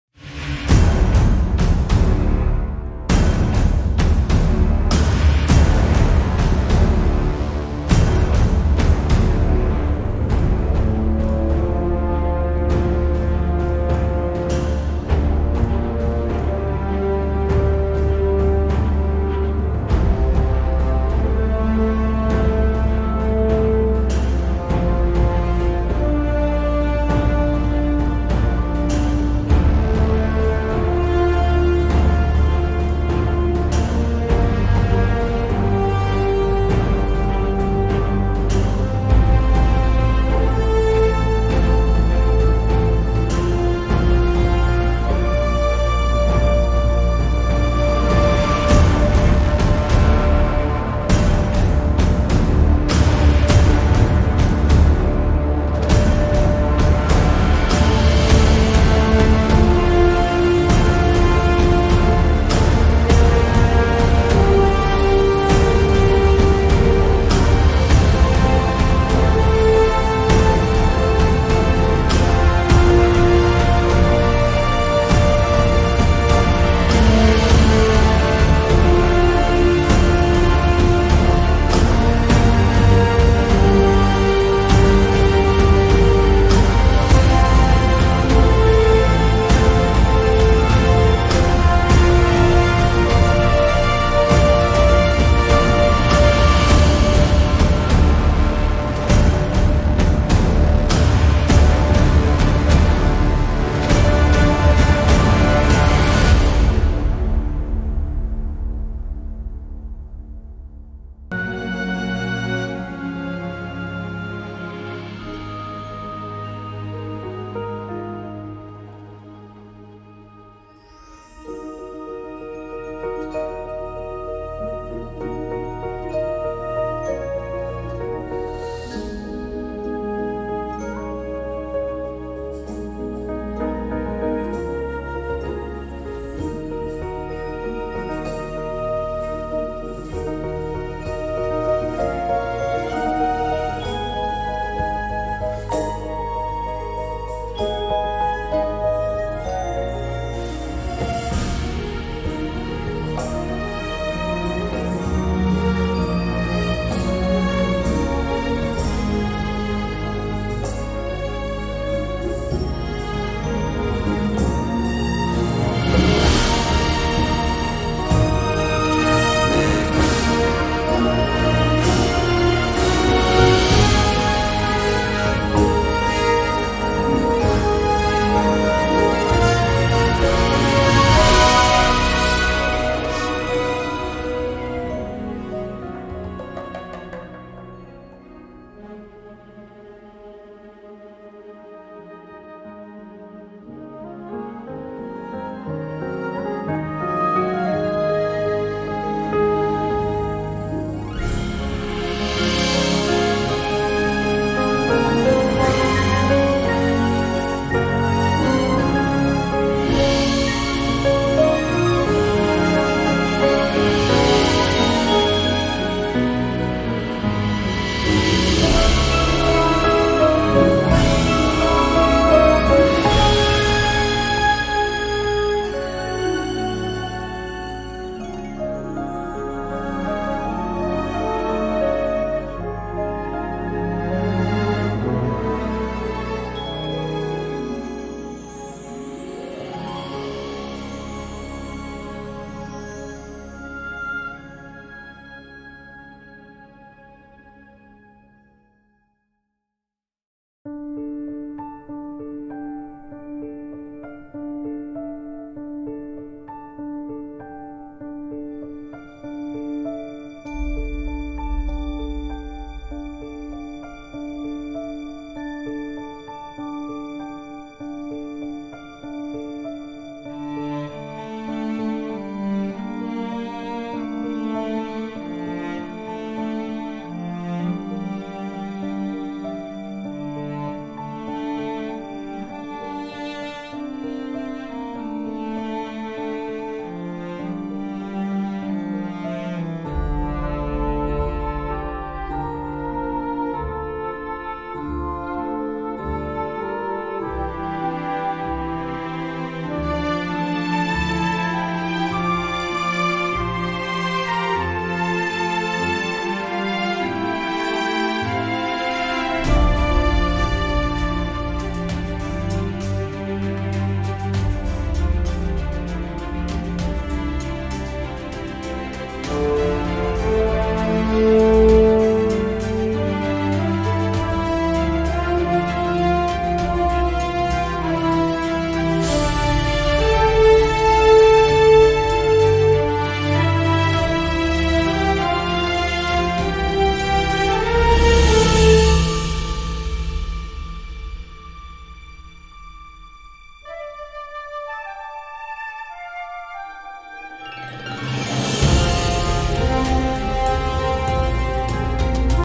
Viking History Quiz - Test Your Knowledge on Norsemen and Their Legendary Era! Part 1
Dive into the fascinating world of the Vikings with our Viking history quiz! Discover how much you really know about these legendary Norsemen, their epic voyages, fierce battles, and rich culture. This quiz covers key aspects of Viking history, from their famous raids and exploration to their daily life. Perfect for history buffs, students, and anyone interested in the Viking Age. Test your knowledge and challenge your friends!